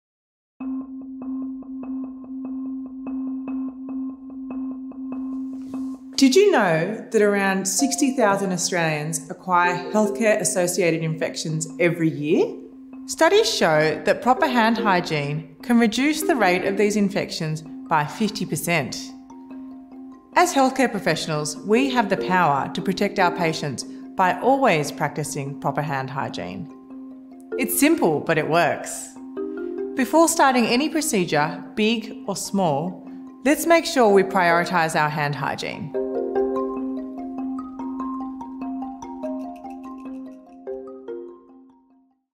Hand Hygiene Moments - Before any procedure
In this video, Dr Phoebe Holdenson Kimura a Medical Advisor for the Commission and GP, highlights the second moment for hand hygiene.

Discover why practising good hand hygiene before any procedures is a fundamental component of patient safety and infection control.